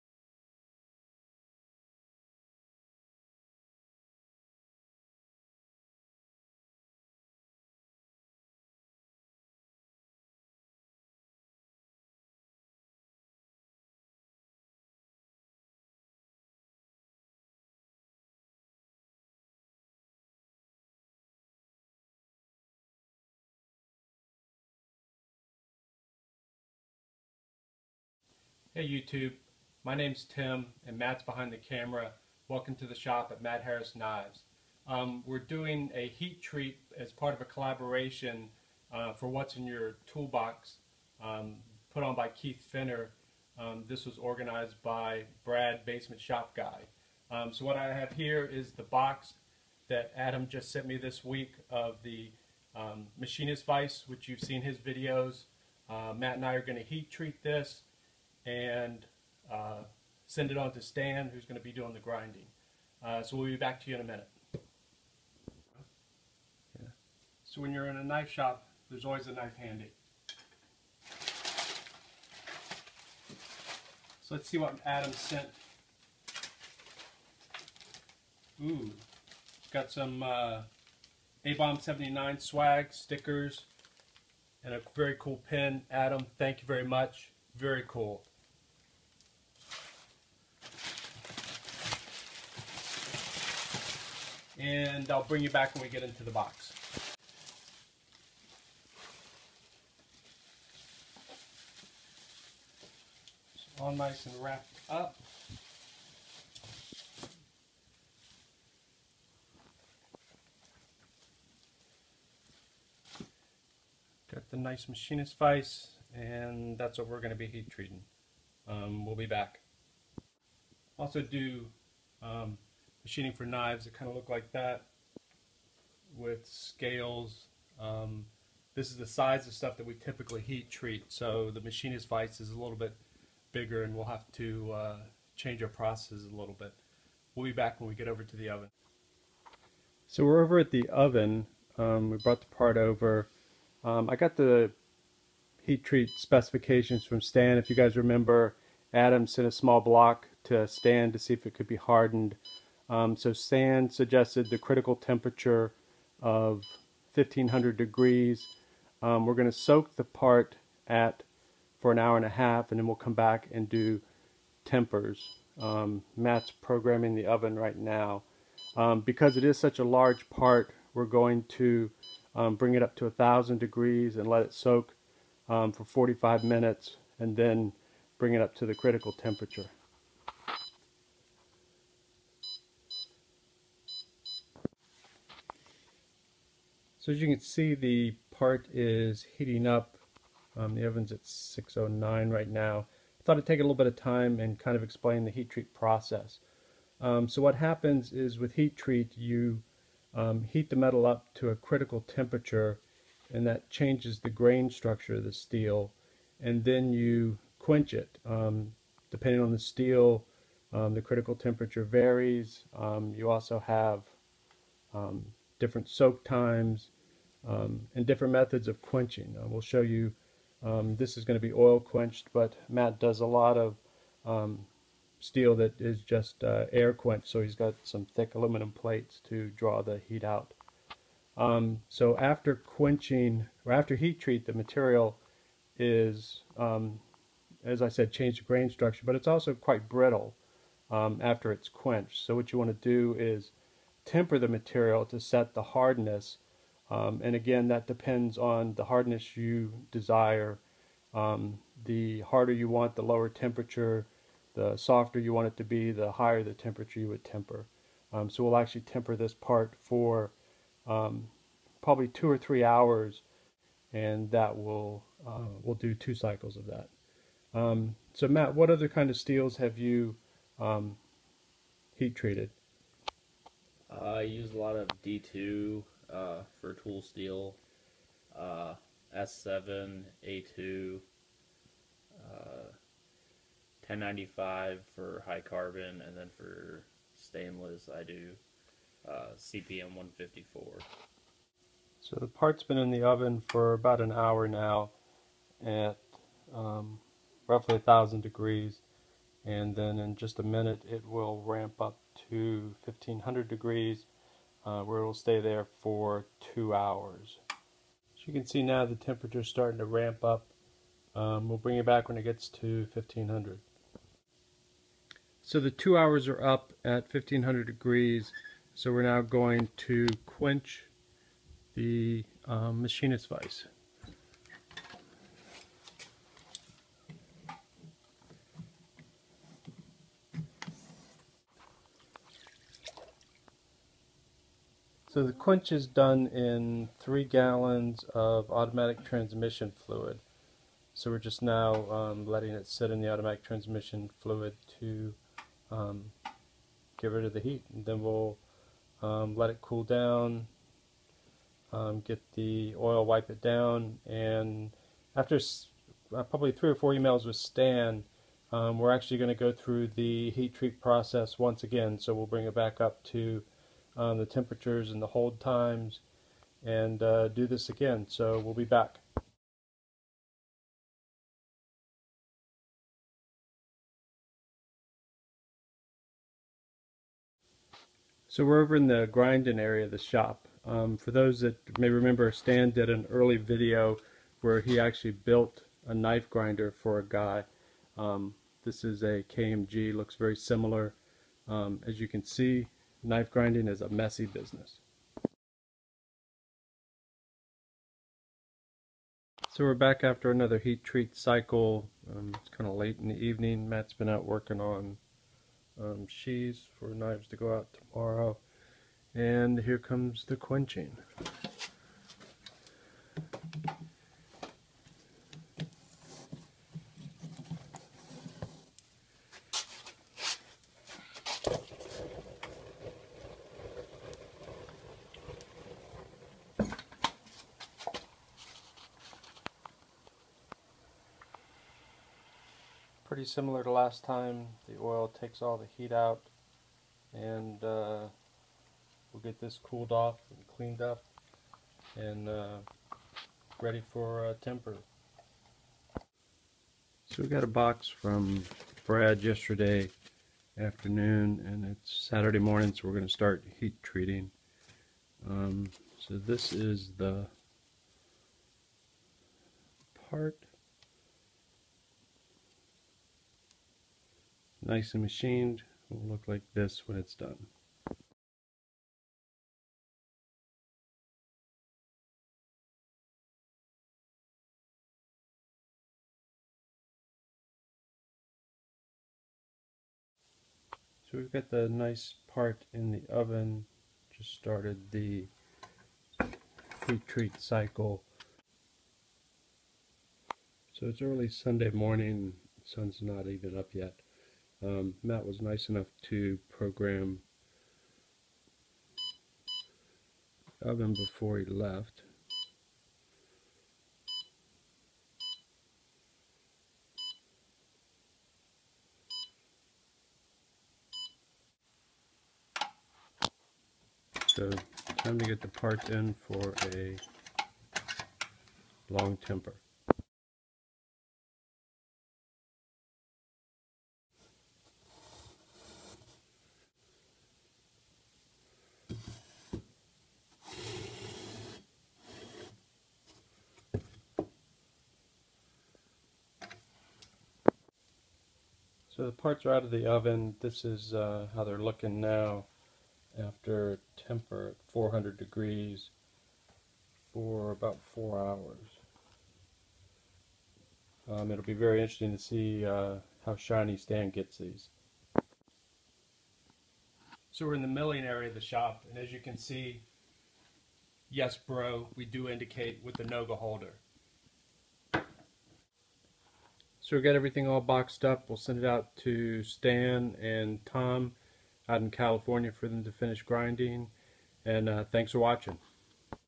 WIYB - Machinist Vise Heat Treat
Welcome to the shop.  This video is part of a series of videos documenting a machinist vise which is being donated to Kieth Fenner's What's in Your Box effort.  We heat treated three parts: a hemispherical washer made by John Saunders, the vise body made by Adam Booth, and the vise jaw made by Brad Jacobs.